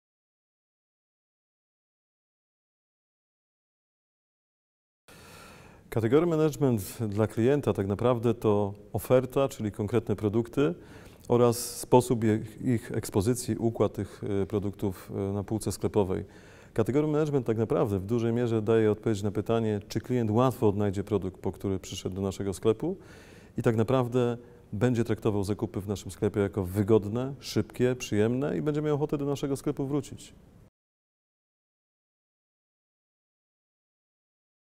Category management: co to jest? - Roman Szymanda
Na pytanie czym jest category management oraz jakie znaczenie ma dla klienta odpowiada Roman Szymanda, kierownik Category Management sieci Intermarché.